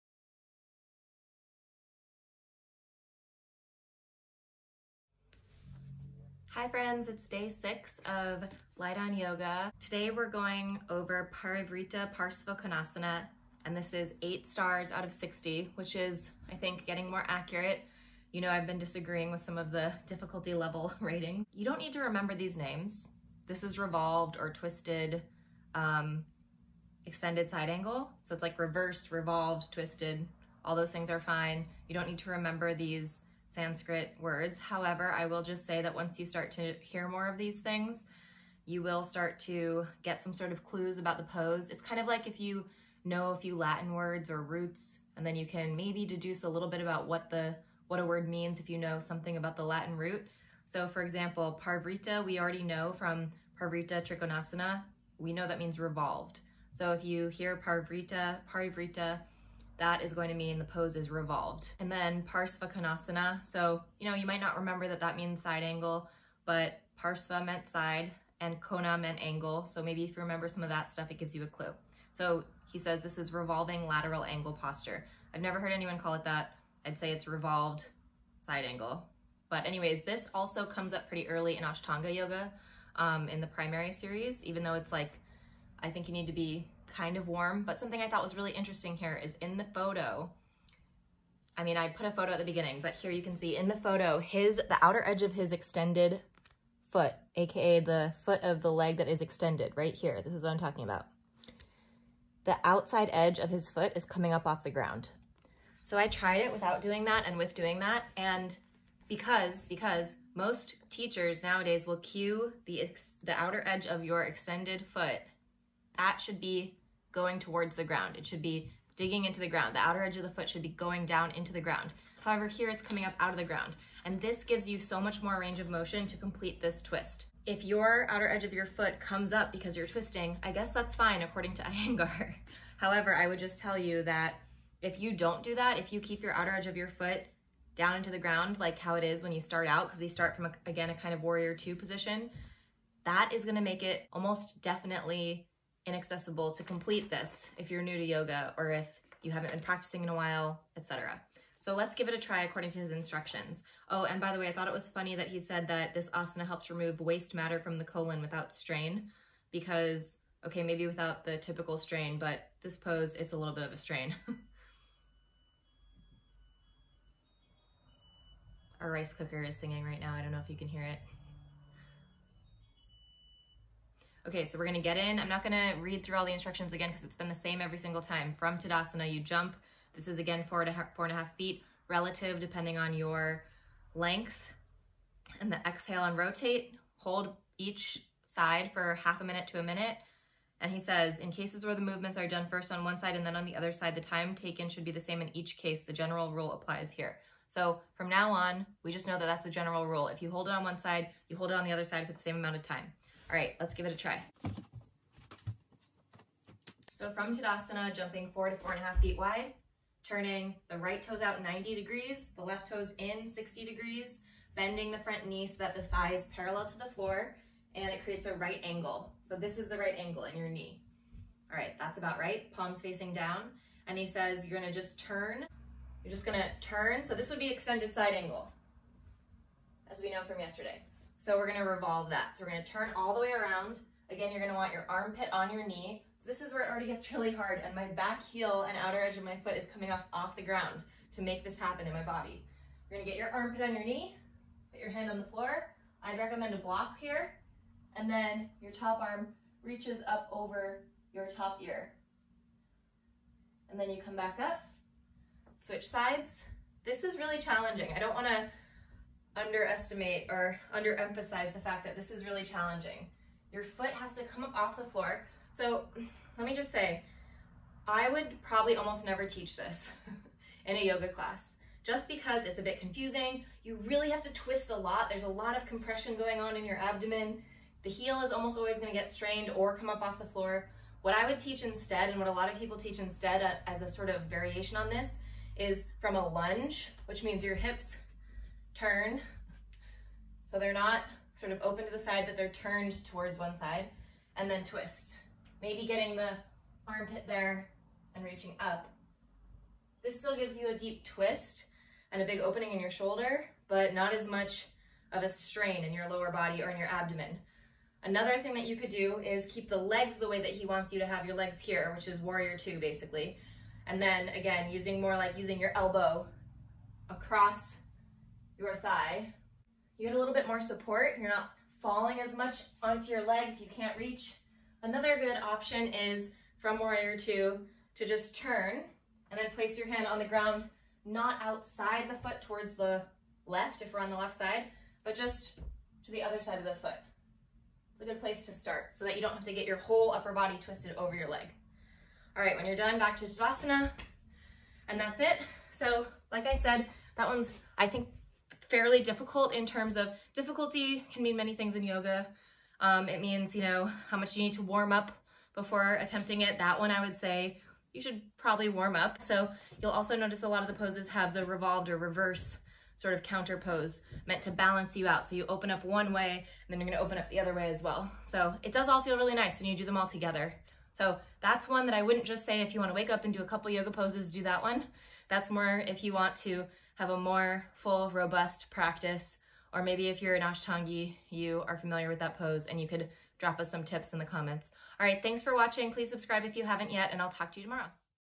#6: Parivrtta Parsvakonasana or Revolved Side Angle| Light on Yoga Asana Explained by a Yoga Teacher
Light on Yoga is considered by most to be the most important yoga asana book. It's on almost every yoga teacher training required reading list.

I am an E-RYT 200 yoga teacher and I have a master's degree in Library & Information Science. In this series, I will go over the yoga asana (or yoga postures / poses) covered in Light on Yoga, one by one.

This is yoga asana / pose / posture #6: Parivrtta Parsvakonasana (also called Revolved Side Angle)

In this video I'm wearing:
-Align yoga pants

THE YOGA EQUIPMENT I USE & RECOMMEND
Travel version
Yoga towel
Yoga book
Meditation book
Lavender Essential Oil